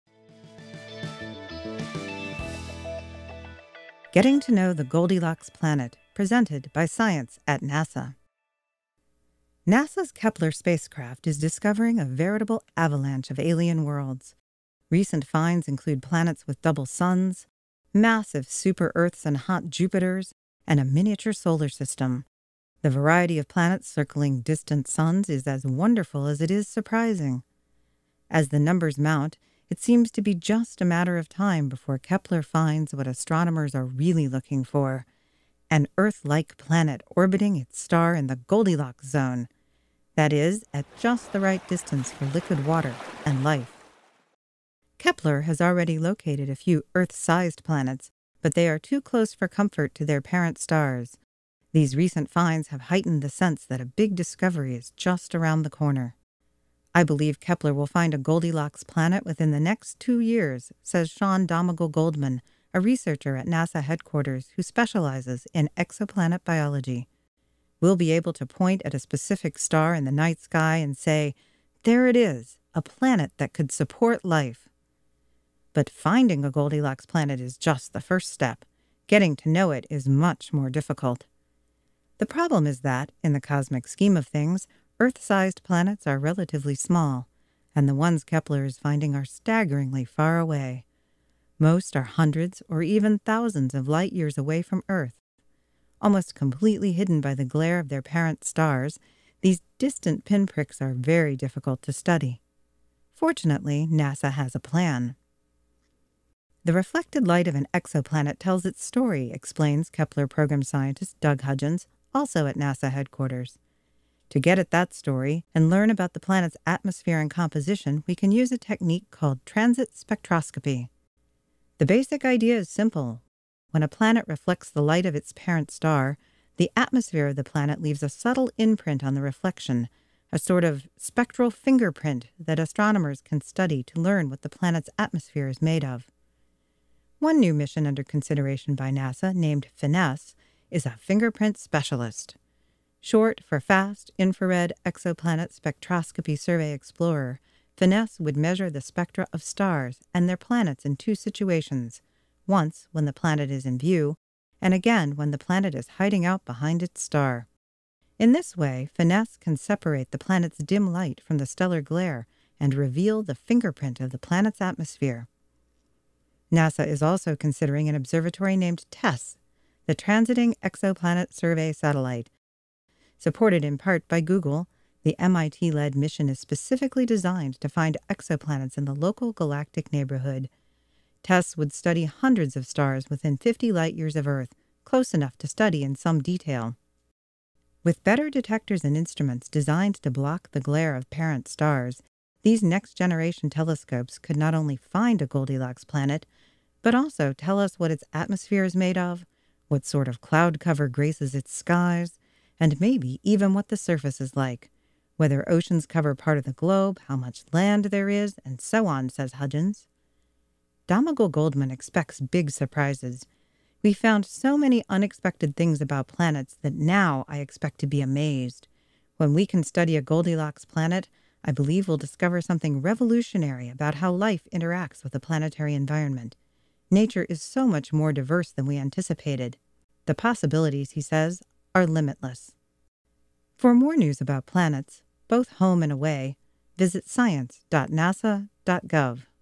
ScienceCasts: Getting to Know the Goldilocks Planet
NASA's Kepler spacecraft is discovering a veritable avalanche of alien worlds. As the numbers mount, it seems to be just a matter of time before Kepler finds what astronomers are really looking for: an Earth-like planet orbiting its star in the "Goldilocks zone".

NOTE: The TESS mission was selected to move into the formulation stage in the spring of 2013.